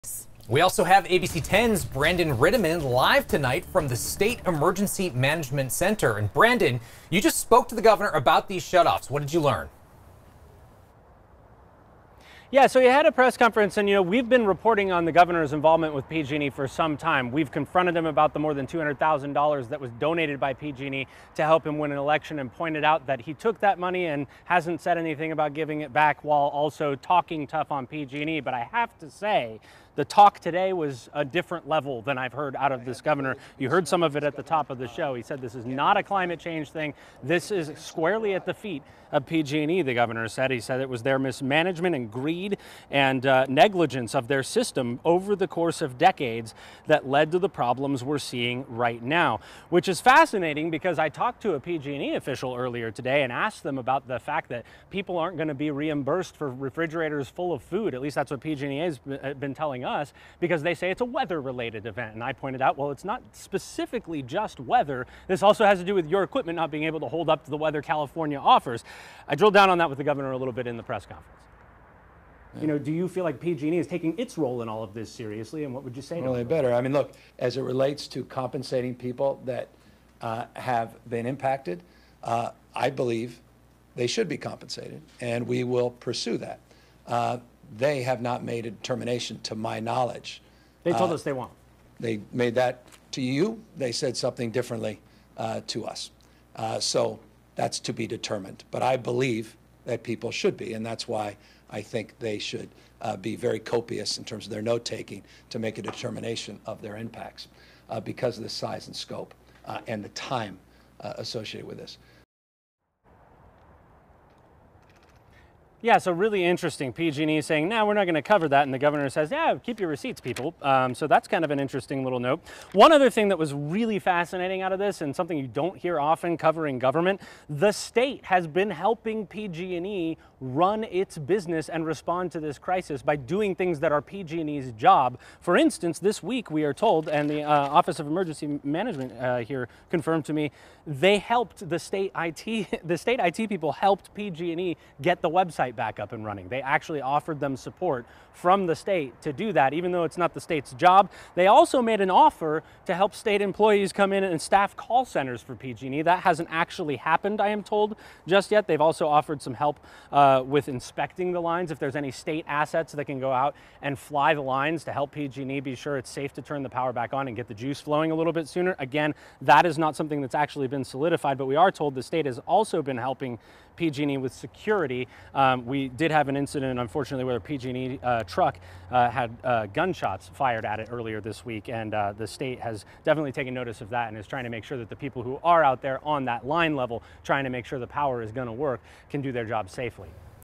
PG&E power shutoff | Recap of Gov. Newsom's Thursday press conference about the shutoffs
Gov. Newsom talks to reporters about the PG&E power shutoffs. ABC10's Brandon Rittiman was there, and has a recap of what was said and implied.

Sacramento News